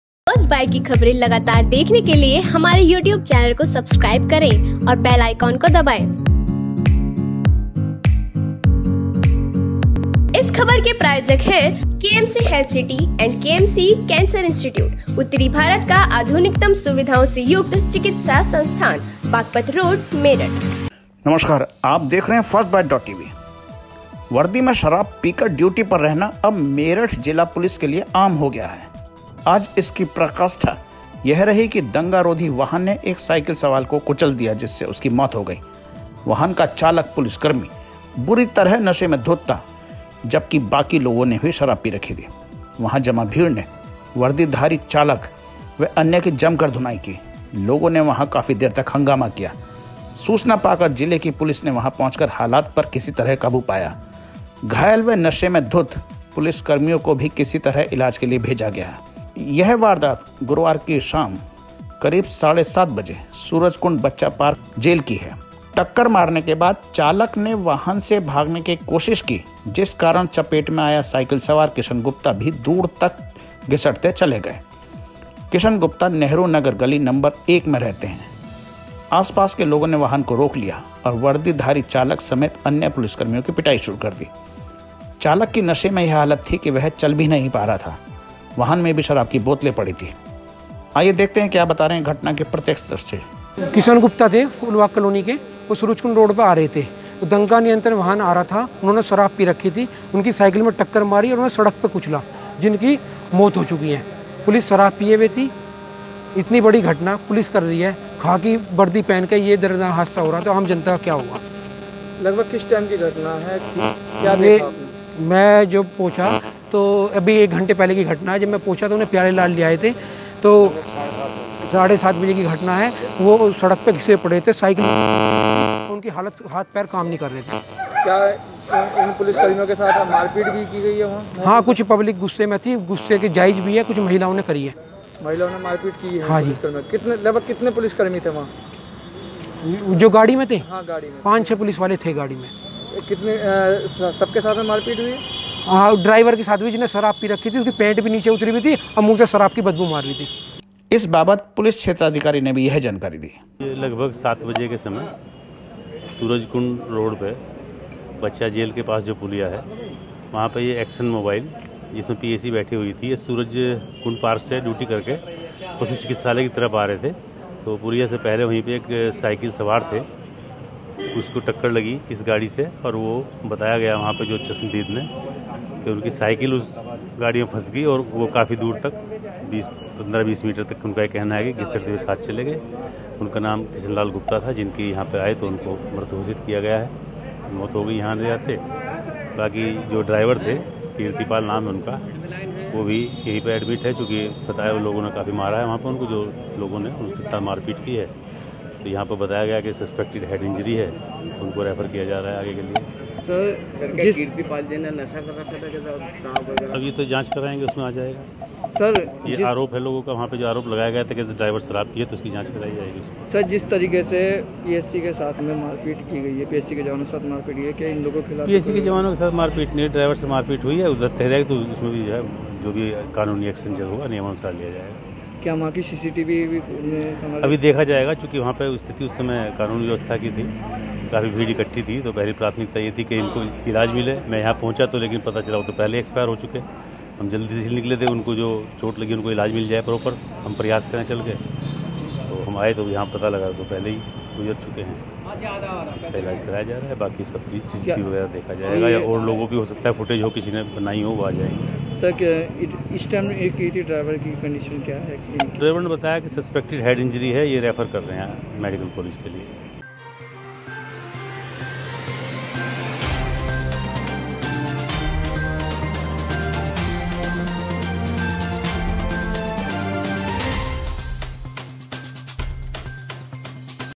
नशे में धुत्त दंगा रोगी वाहन के चालक ने साइकिल सवार को रौंदा, भीड़ ने नशेबाज पुलिस को पीटा
नशे में धुत्त दंगा रोगी वाहन के चालक ने साइकिल सवार को रौंदा, भीड़ ने वाहन में तोड़फोड़ की, नशेबाज पुलिस को पीटा